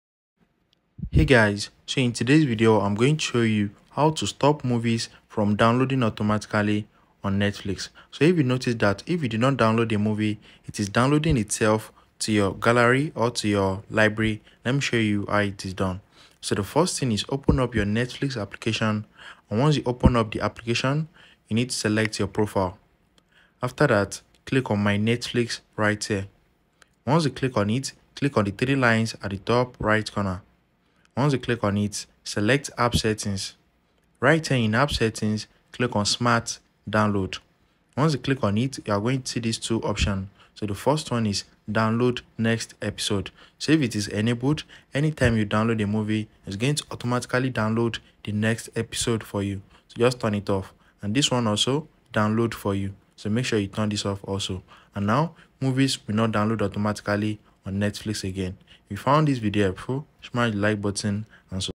How to Stop Movies from Downloading Automatically on Netflix
Tired of Netflix automatically downloading the next episode of your favorite show? This tutorial will show you how to disable this feature and control your downloads manually. Learn about the steps involved and how to manage your Netflix download settings to your preference. Watch now to prevent unwanted downloads and save storage space on your device!